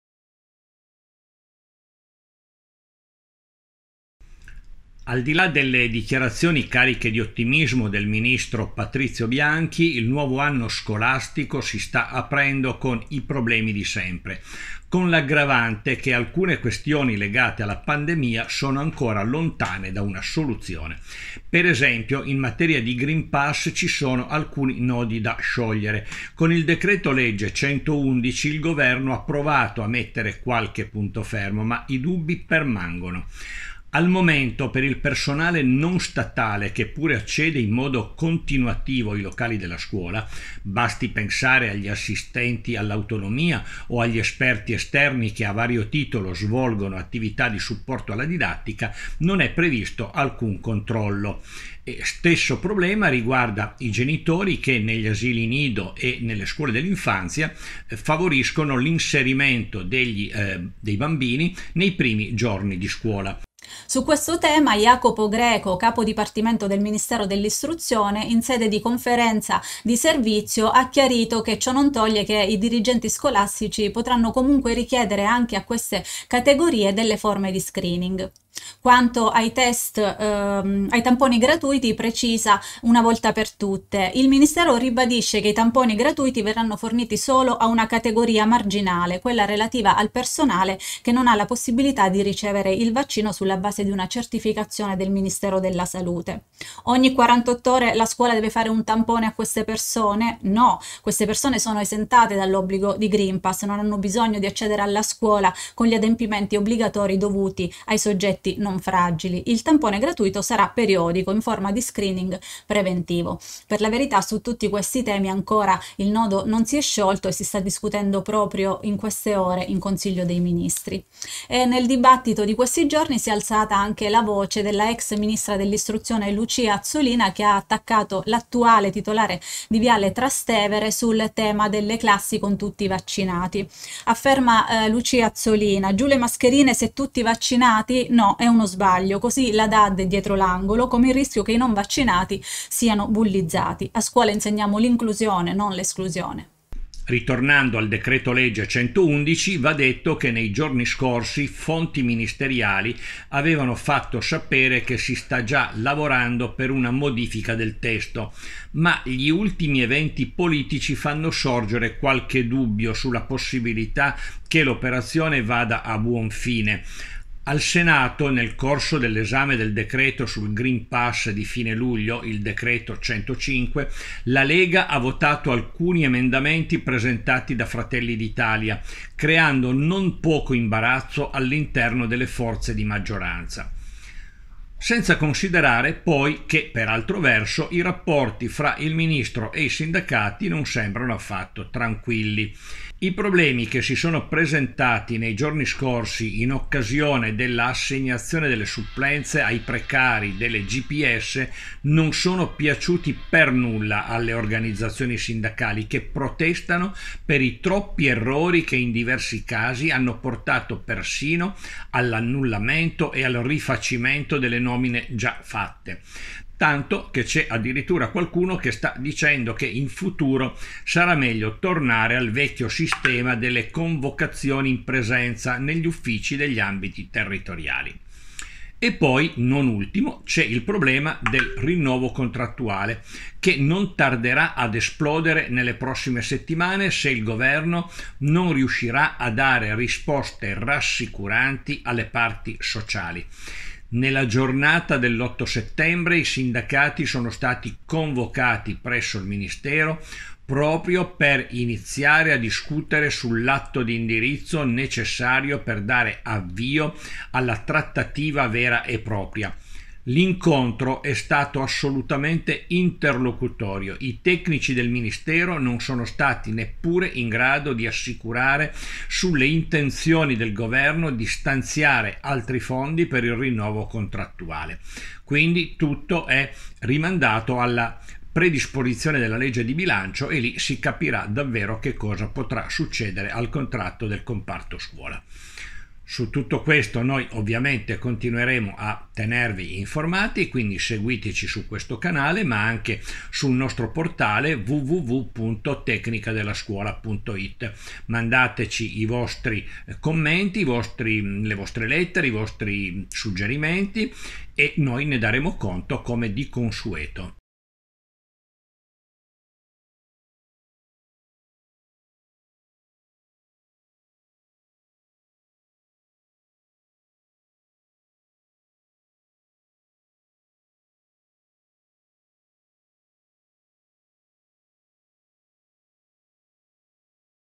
Anno scolastico nuovo, problemi vecchi, si cercano soldi per il contratto scuola
inizioscuola tecnicadellascuola Al di là le dichiarazioni cariche di ottimismo del ministro Patrizio Banchi, il nuovo anno scolastico si sta aprendo con i problemi di sempre, con l’aggravante che alcune questioni legate alla pandemia sono ben lontane da una soluzione. Di tutto questo e altro si parla nel nostro video settimanale.